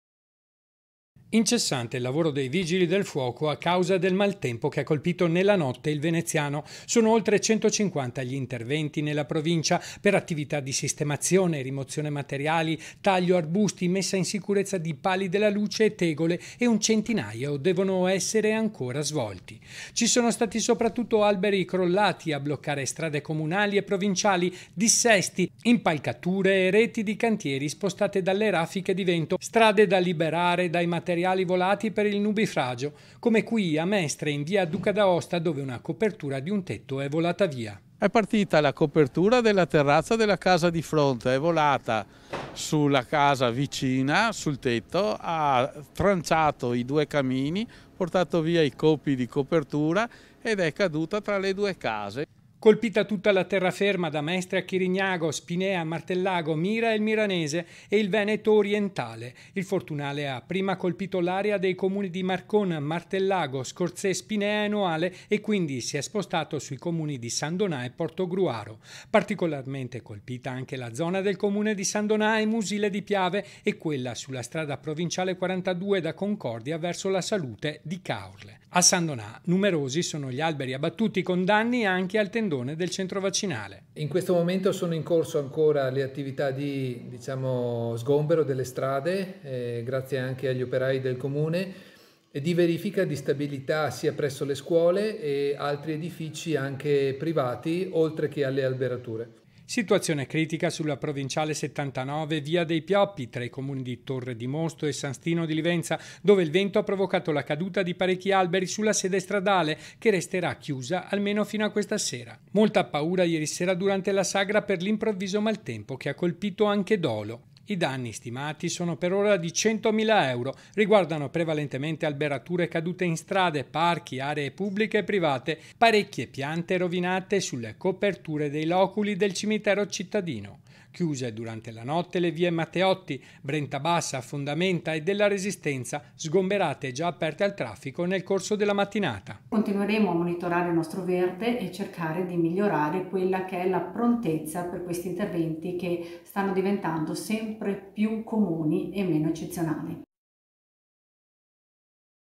MALTEMPO: OLTRE 200 INTERVENTI NEL VENEZIANO | 17/08/2021
A3 NEWS Veneto 17/08/2021 - VENEZIA - Raffiche di vento e pioggia, centinaia gli interventi dei Vigili del Fuoco nel territorio del veneziano.   || Incessante il lavoro dei Vigili del Fuoco per il maltempo che ha colpito nella notte il veneziano. Sono oltre 150 gli interventi nella provincia di Venezia, per attività di sistemazione, rimozione materiali, taglio arbusti, messa in sicurezza di pali della luce e tegole, e un centinaio devono essere ancora svolti. Ci sono stati soprattutto alberi crollati a bloccare strade comunali e provinciali, dissesti, impalcature e reti di cantiere spostate dalle raffiche di vento, strade da liberare dai materiali volati per il nubifragio. Come qui a Mestre in via Duca D'Aosta dove una copertura di un tetto è volata via.Colpita tutta la terraferma, da Mestre a Chirignago, Spinea, Martellago, Mira e il Miranese, e il Veneto orientale. Il fortunale ha prima colpito l’area dei Comuni di Marcon, Martellago, Scorzè, Spinea e Noale e quindi si è spostato sui Comuni di San Donà e Portogruaro. Particolarmente colpita anche la zona del Comune di San Donà e Musile di Piave e quella sulla strada provinciale 42 da Concordia verso la Salute di Caorle. A San Donà numerosi alberi abbattuti con danni anche al tendone del centro vaccinale.  Situazione critica sulla provinciale 79 via dei Pioppi tra i Comuni di Torre di Mosto e San Stino di Livenza dove il vento ha provocato la caduta di parecchi alberi sulla sede stradale che resterà chiusa almeno fino a questa sera.Molta paura ieri sera durante la sagra per l'improvviso maltempo che ha colpito anche Dolo.I danni - stimati per ora in circa 100mila euro - riguardano prevalentemente alberature cadute in strade, parchi, aree pubbliche e private. In alcuni casi sono stati danneggiati anche tralicci e cavi elettrici e telefonici. Tre gli alberi secolari abbattuti dalla furia del vento nel Parco di Villa Concina, parecchie piante rovinate sulle coperture dei loculi del cimitero cittadino. Chiuse durante la notte le vie Matteotti, Brentabassa, Fondamenta e della Resistenza, sgomberate e già riaperte al traffico nel corso della mattinata. - Intervistati ANDREA CERESER (Sindaco San Donà di Piave), MARTINA VESNAVER (Sindaco Spinea) (Servizio di Nicola Marcato) - Segui Antenna Tre anche sul digitale terrestre! Visita il sito